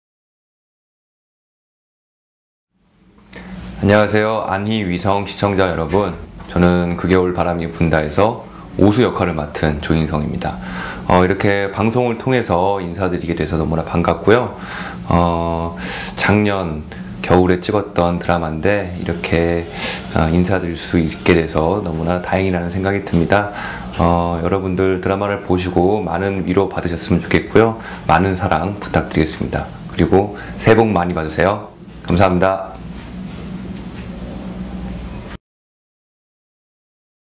Jo In Sung Greetings for Anhui TV Audience
Jo In Sung recorded a clip and gave greetings to Anhui TV audience asking everyone to support TWTWB :)